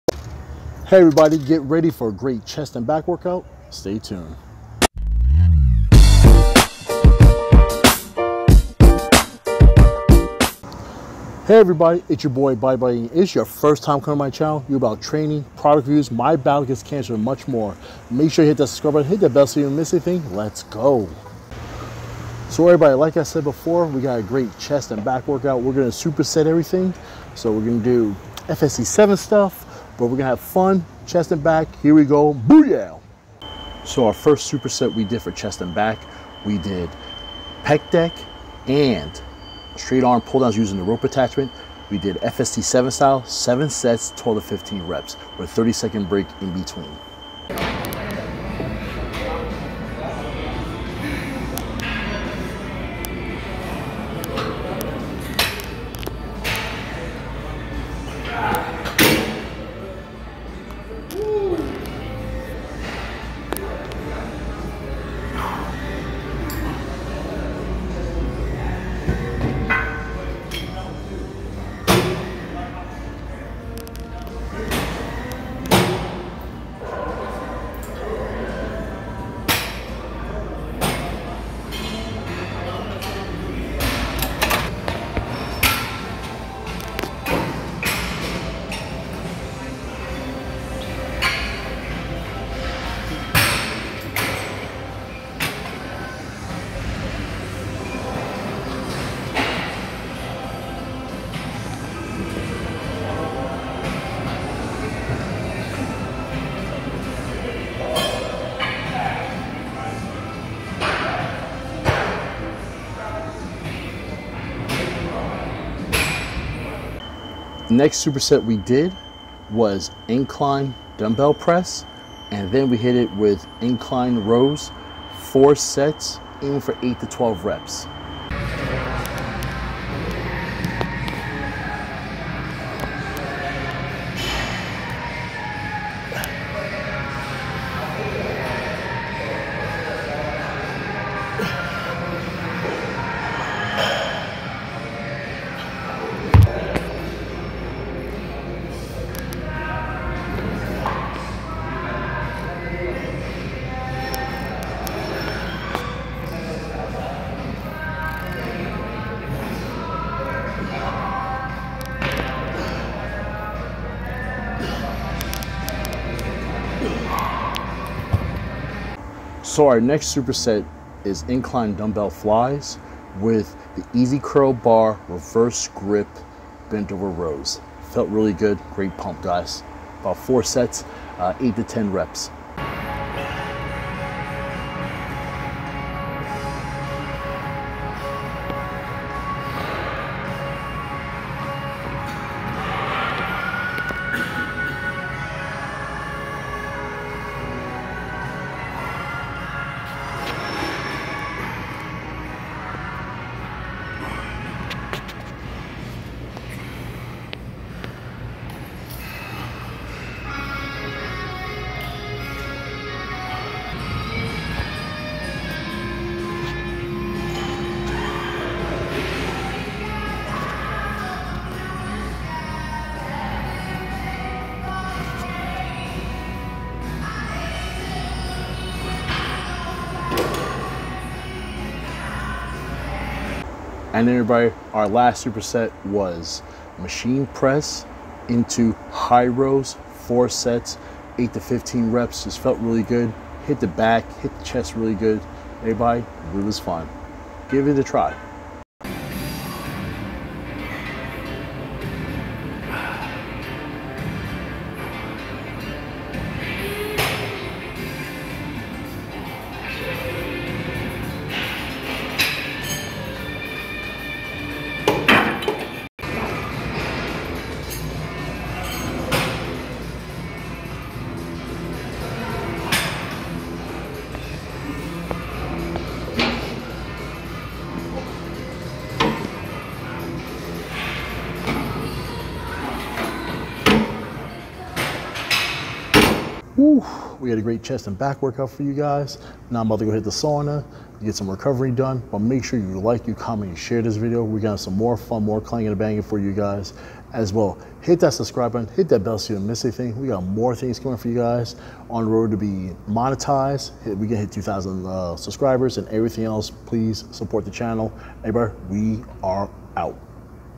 Chest & Back Workout
Hi everyone, I bring to you a great Clanging and Banging show hitting Chest and Back. I superset chest and back.

Brands, companies and fans I hope to hear from you.

or